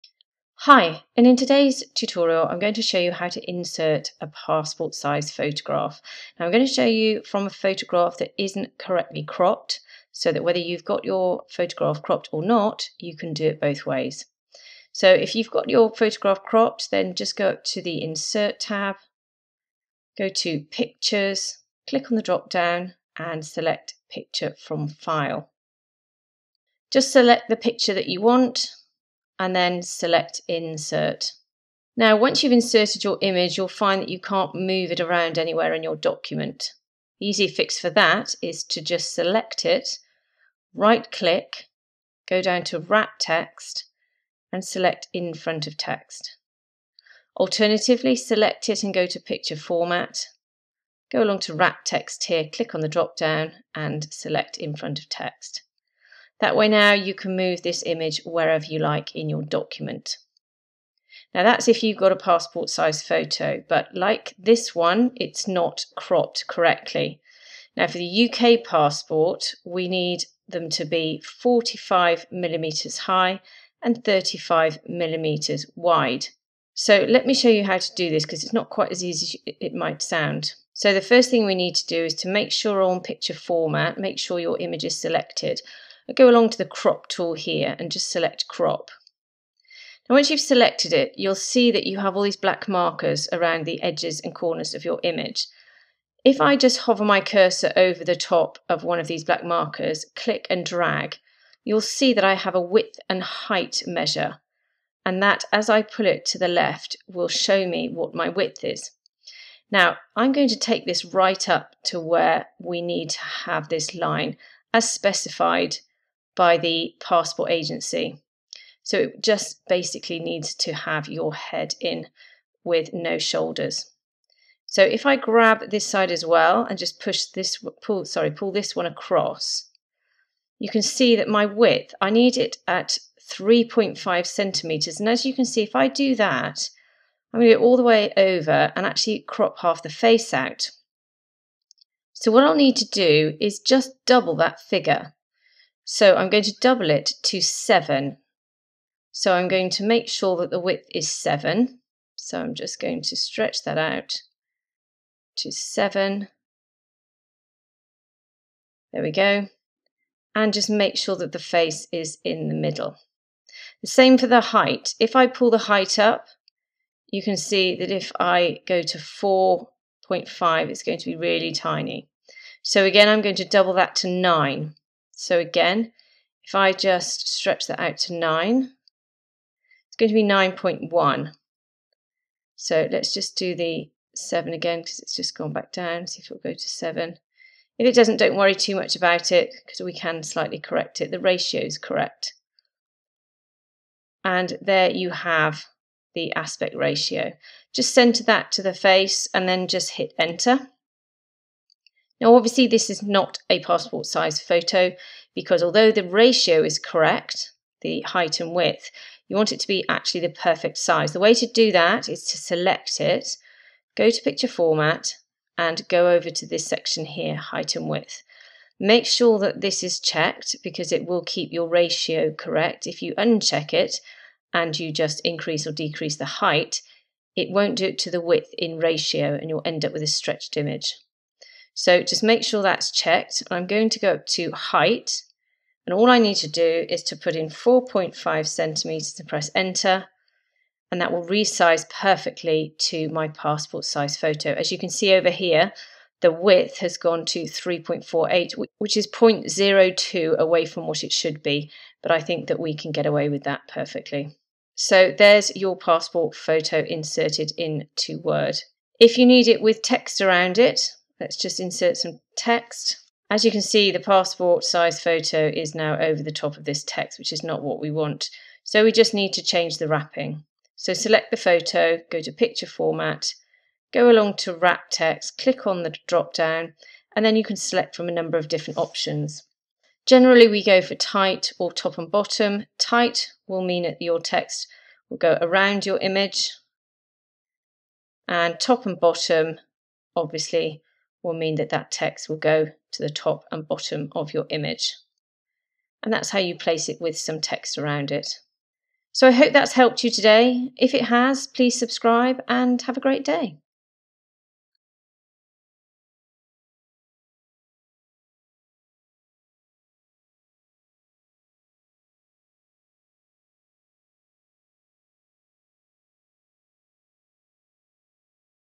How to insert a PASSPORT SIZE photo into word | CROP AND RESIZE an image in word
Learn how to insert a passport size photo into word with this simple tutorial.  Even if your photo is the wrong size or proportions the tutorial will teach you how to crop your image to the correct dimensions, move it around your document and place it within text.

🎤 Microphone Stand/Holder
🖱 Mouse - Wacom Pen